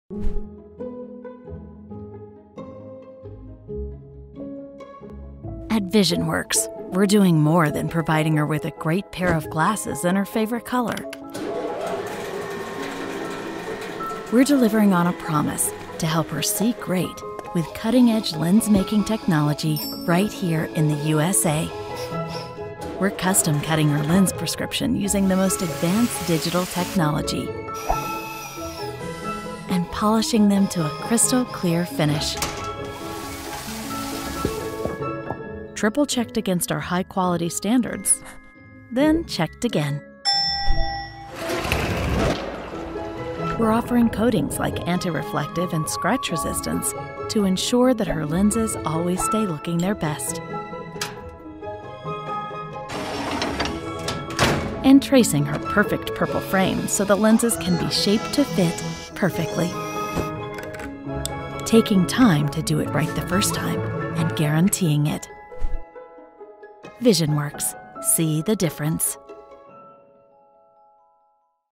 Visionworks | See The Difference
Visionworks lens making process.